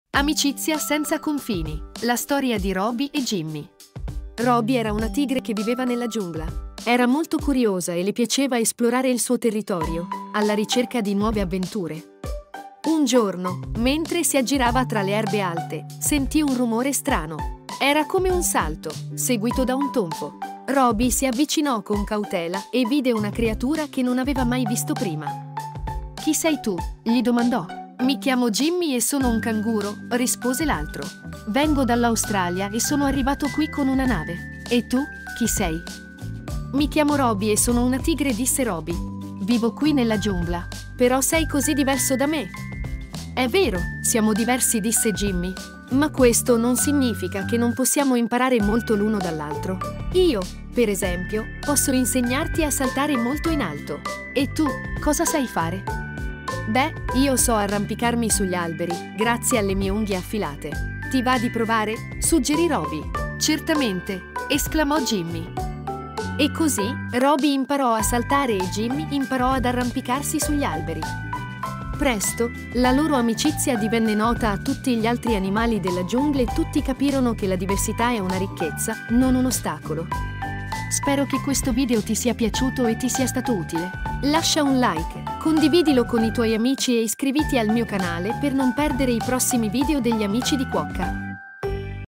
La diversità: la storia di Roby la tigre e Jimmy il canguro
Ciao! Questa è l'avventura di Roby la tigre e Jimmy il canguro. Scopri cosa stanno per apprendere sulla diversità... condividi con i tuoi amici!!!